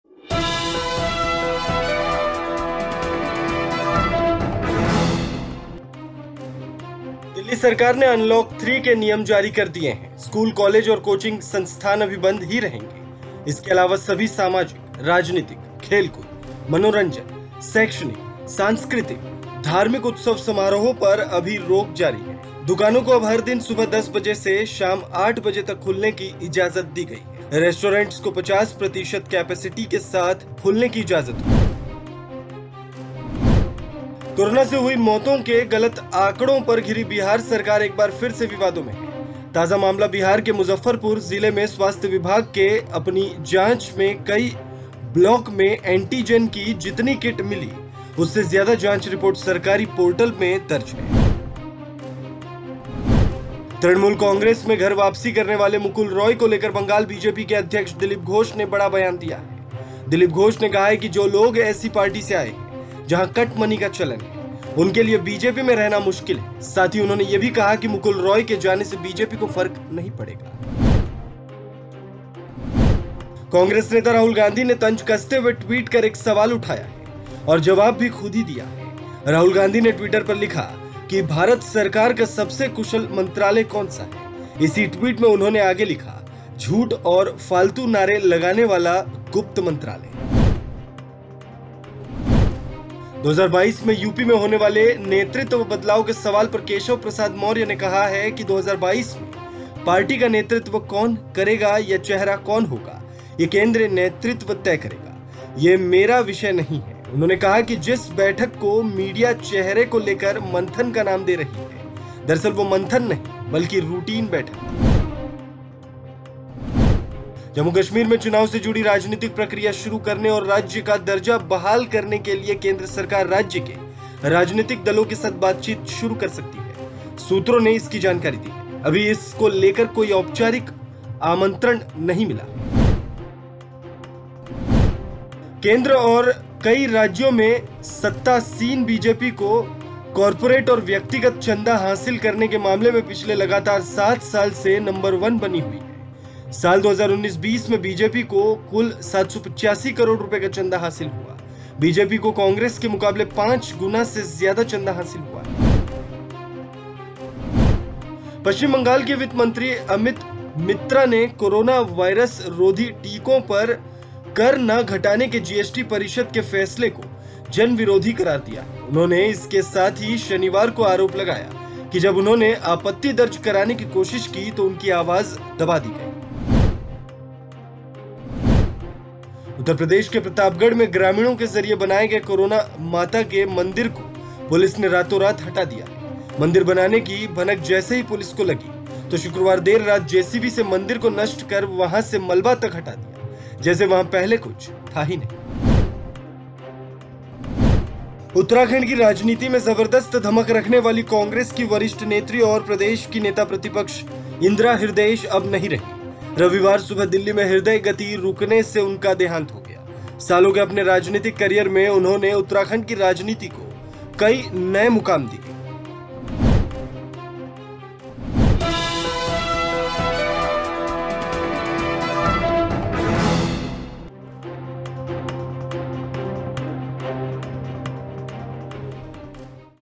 Delhi Unlock 3: दिल्ली में सोमवार से क्या खुलेगा, क्या रहेगा बंद? जानिए Guidelines | वनइंडिया हिंदी
Delhi Unlock 3: What will be open in Delhi from Monday, what will remain closed? Know Guidelines. The Delhi government has issued the rules of Unlock 3. Schools-colleges and coaching institutes will remain closed for now. Apart from this, all social, political, sports, entertainment, educational, cultural, religious festival celebrations are also banned. Shops have now been allowed to open every day from 10 am to 8 pm. Restaurants will be allowed to open with 50% capacity.

Delhi सरकार ने Unlock 3 के नियम जारी कर दिए हैं. स्‍कूल-कॉलेज और कोचिंग संथान अभी बंद ही रहेंगे. इसके अलावा सभी सामाजिक, राजनीतिक, खेलकूद, मनोरंजन, शैक्षणिक, सांस्कृतिक, धार्मिक उत्सव समारोहों पर भी अभी रोक जारी है. दुकानों को अब हर दिन सुबह 10 बजे से शाम 8 बजे तक खुलने की इजाजत दी गई है. रेस्‍टोरेंट्स को 50% कपैसिटी के साथ खुलने की इजाजत होगी.